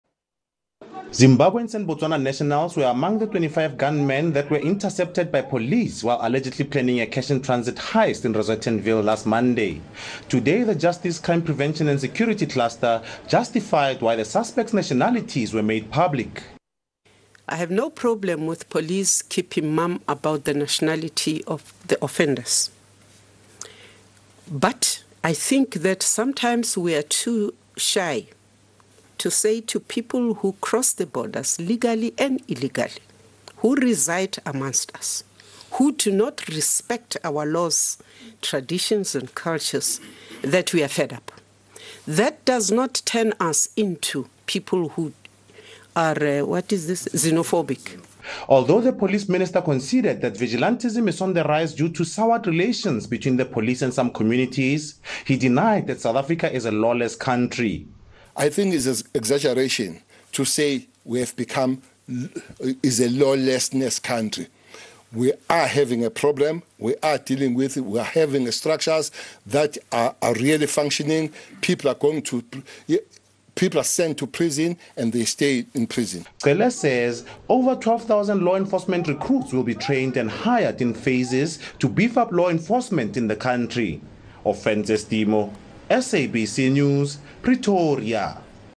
South Africa's law enforcement under the spotlight
Defence Minister Thandi Modise says most crime syndicates in the country are not run by South Africans. She says government is often too shy to call out foreigners when they fail to respect South African laws, out of fear of being labelled xenophobic. Modise was part of the Justice, Crime Prevention and Security Cluster Panel of Ministers that briefed the media in Pretoria on Sunday.